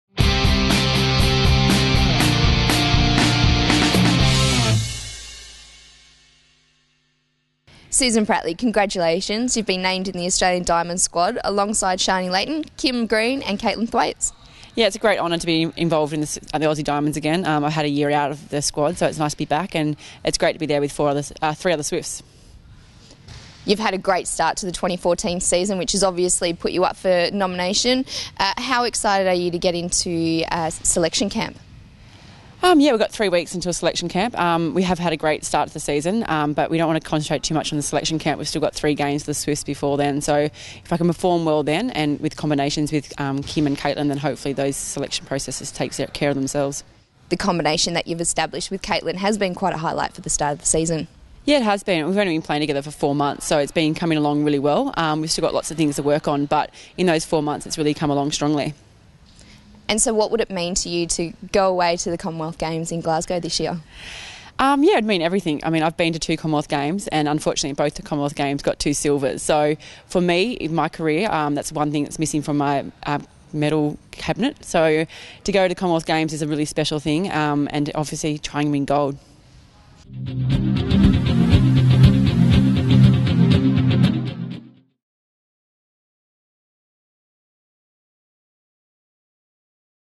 NSWSwiftsTV: Pratley returns to Diamonds squad
Swifts shooter Susan Pratley spoke to NSWSwiftsTV after she was named in the 2014 Australian Diamonds Squad.